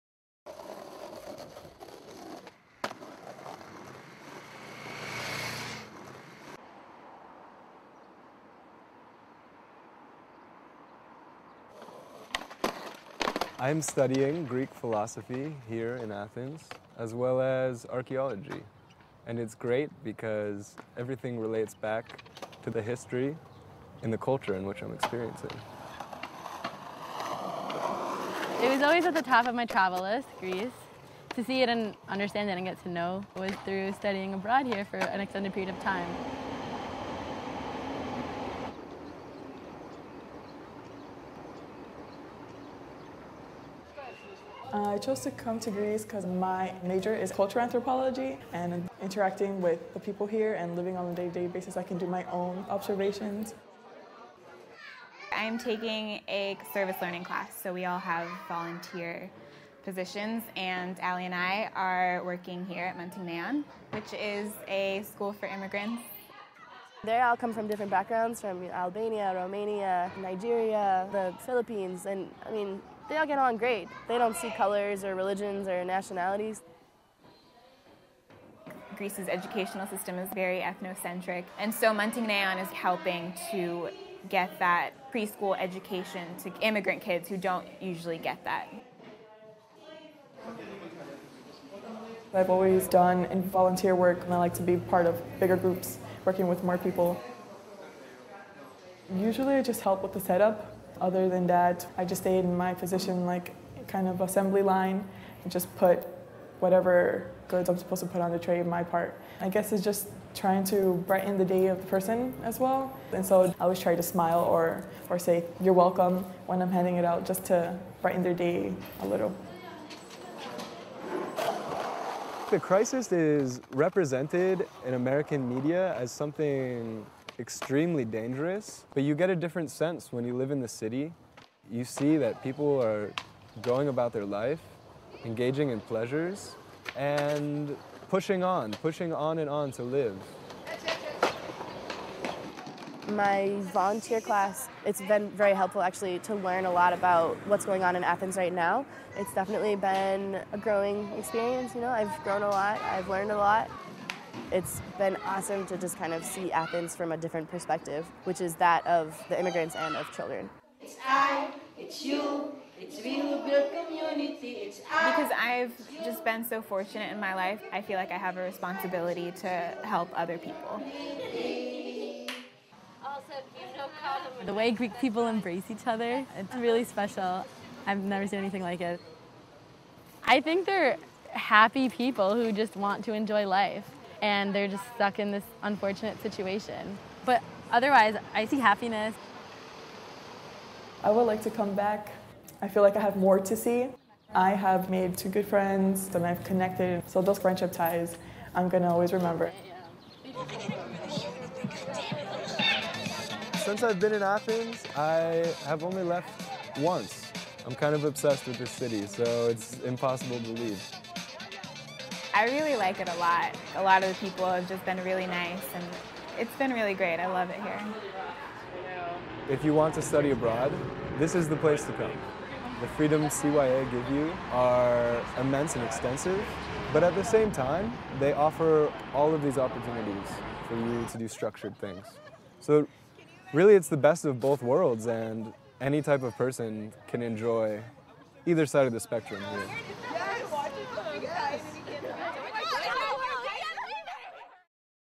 CYA Student Reflections: Greek Culture and Athenian Life
CYA students discuss their perspectives on Greek culture and Athenian life obtained through community interaction, service learning and independent study.